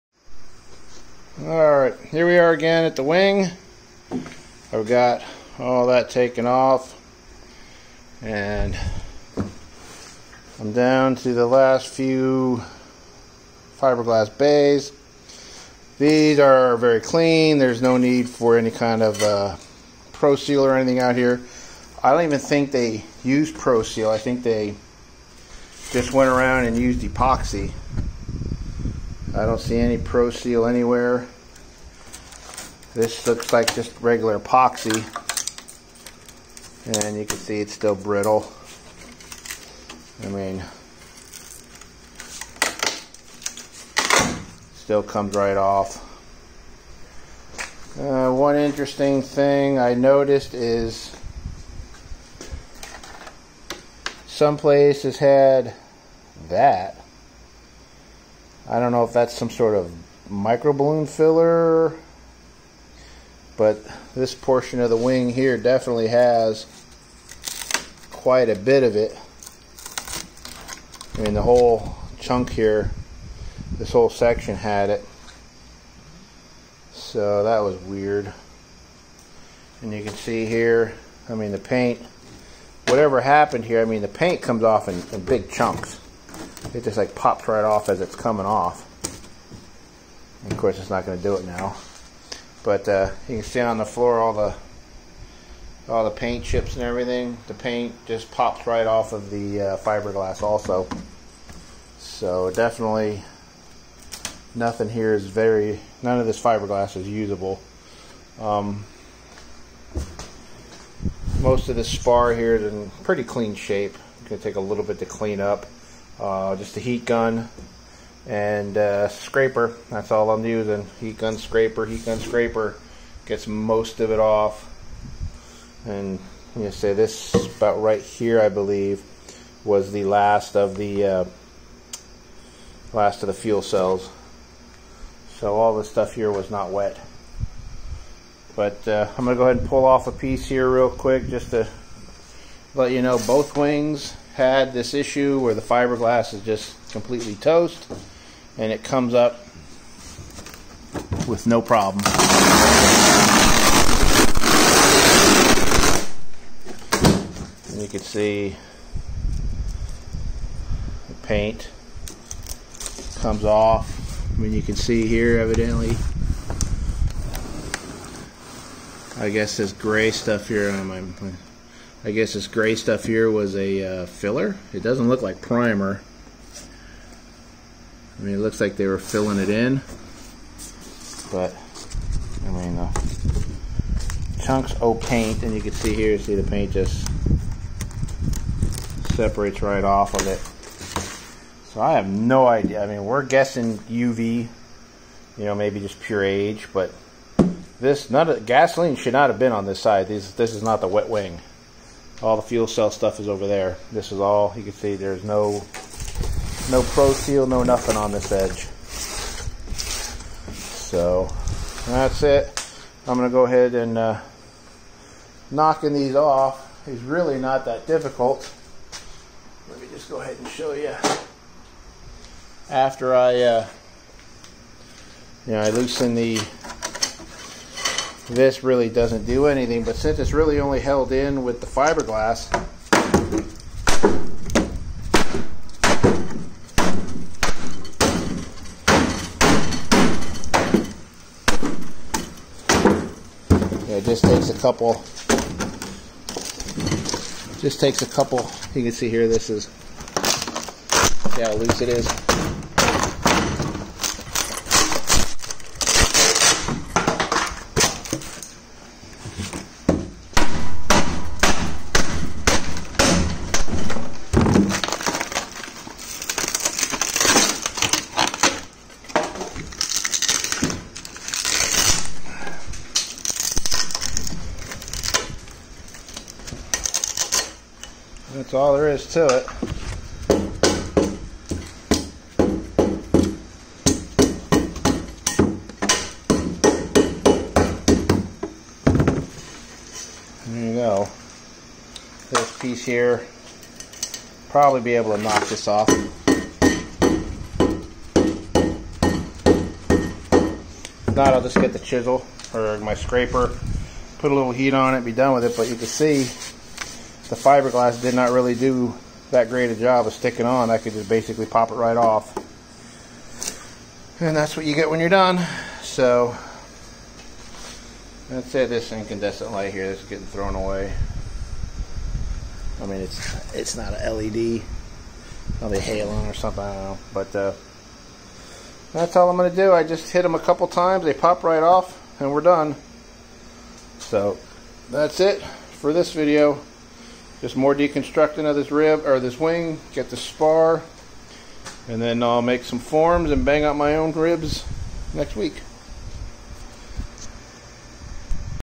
More BD-4B wing deconstruction and main spar recovery.
How I removed the severely damaged fiberglass and metal from the BD-4B wing.  The main purpose is to reccover the spar and reuse it.